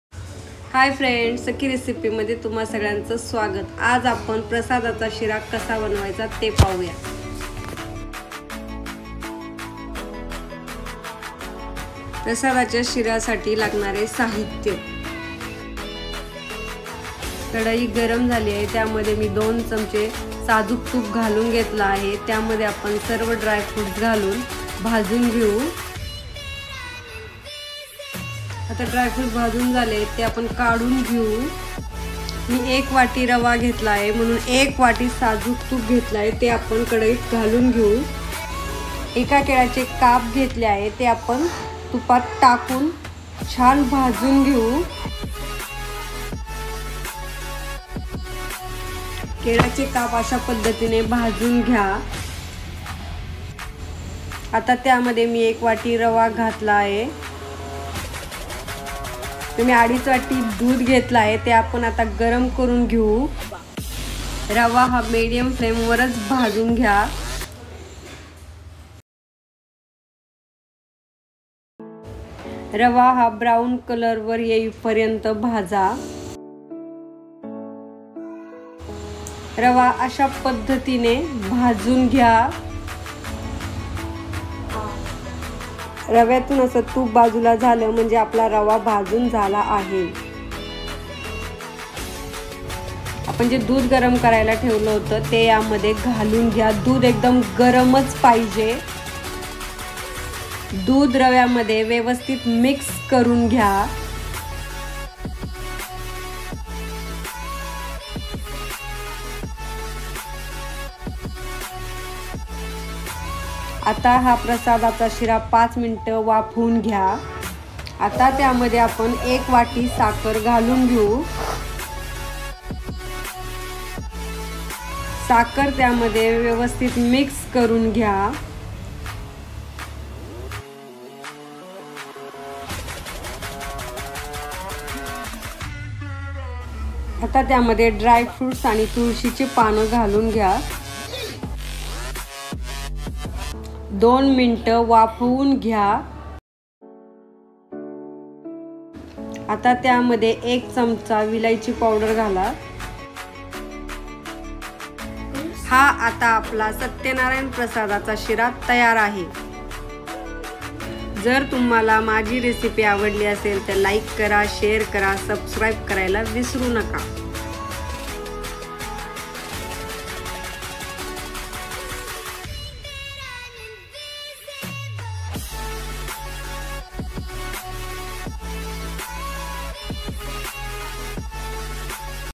Prasad sheera/ Satyanarayan Prasad Sheera#viral
प्रसाद शिरा बनवण्यसाठी लागणारे साहित्य

1 वाटी रवा, 1 वाटी साजूक तूप, 1 वाटी साखर, 1केळ,2 1/2  वाटी दूध, 1/2 चमचा विलायची पावडर,सुका मेवा (काजू, मनुके, बदाम, पिस्ता),